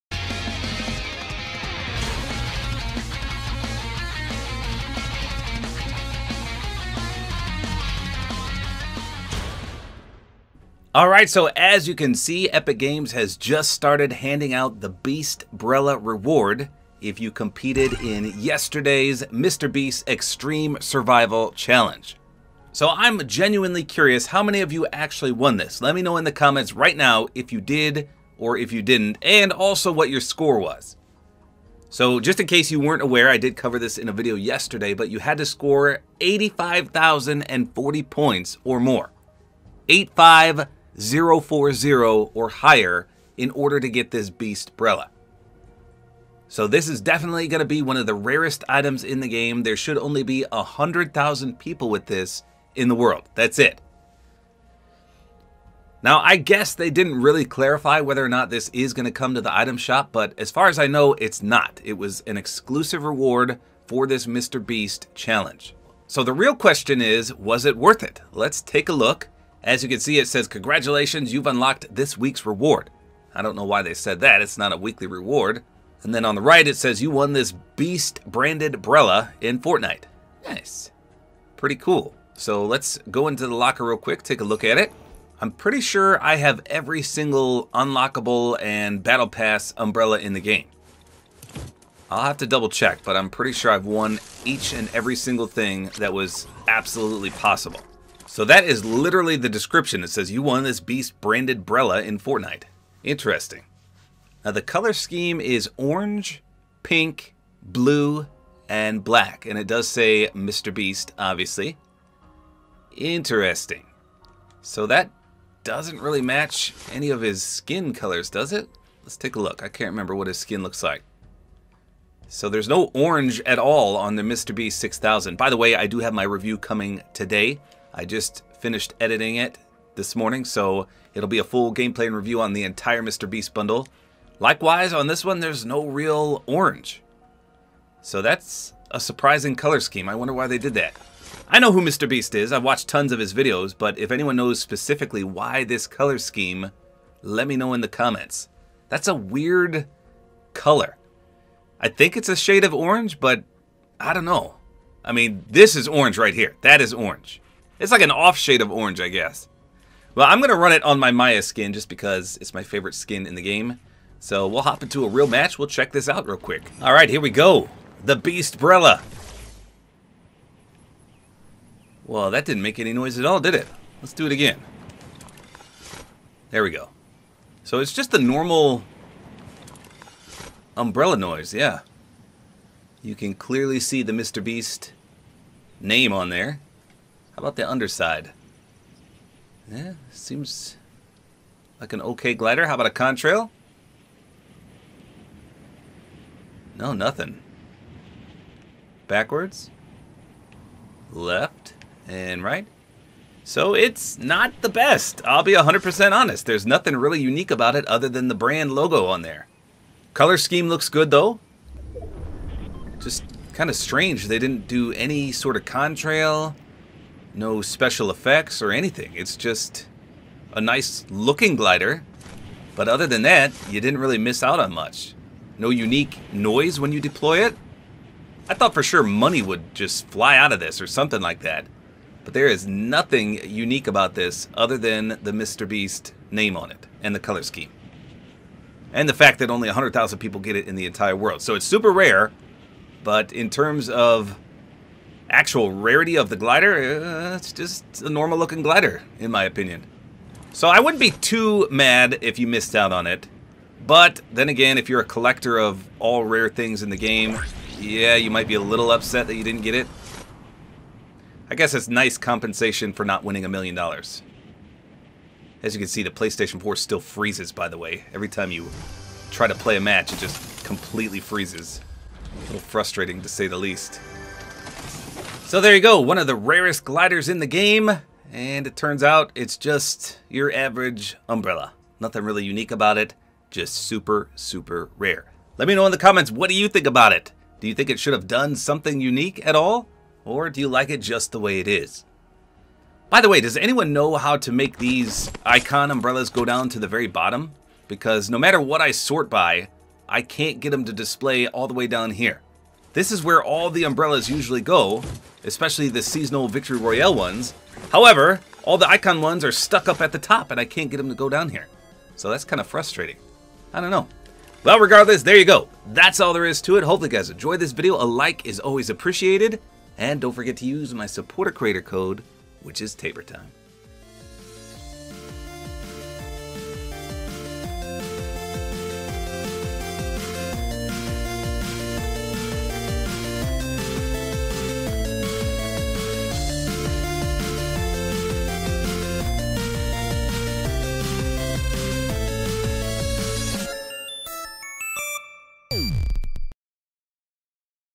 This Is The RAREST Glider In The Game -- Was It Worth Getting? (Beast Brella Gameplay and Review)
Only 100,000 people in the world have this glider, making it most likely the rarest glider in the game.  Let's see how it plays.